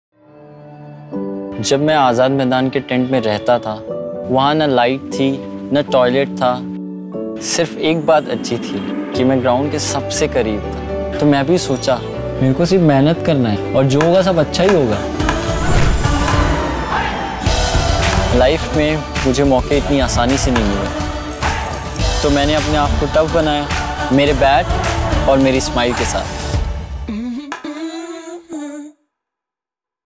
The floodlights of optimism | Yashasvi Jaiswal
Overcoming challenges becomes easy when you have hope. Wankhede floodlights were Yashasvi’s lighthouse of hope. Check out his story.